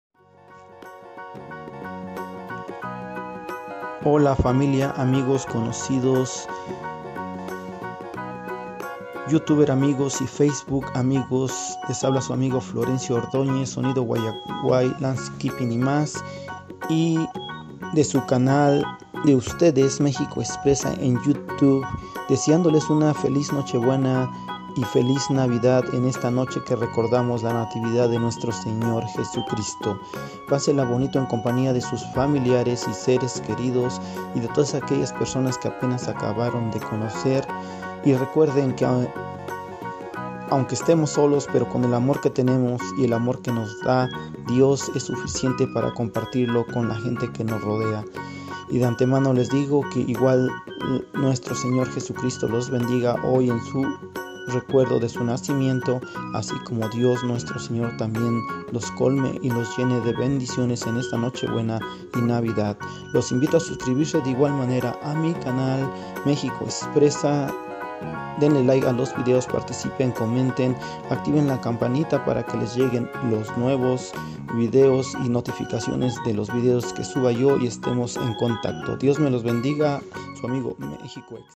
FELIZ NOCHE BUENA Y FELIZ NAVIDAD DE PARTE DE SU CANAL MEXICO EXPRESA December 24, 2019
FELIZ NOCHE BUENA Y FELIZ NAVIDAD 🎄  FAMILIA AMIGOS CONOCIDOS YOUTUBERAMIGOS DIOS LES BENDIGA DE PARTE DE SU AMIGO FLORENCIO ORDÓÑEZ  DE EL CANAL DE USTEDES MEXICO EXPRESA BENDICIONES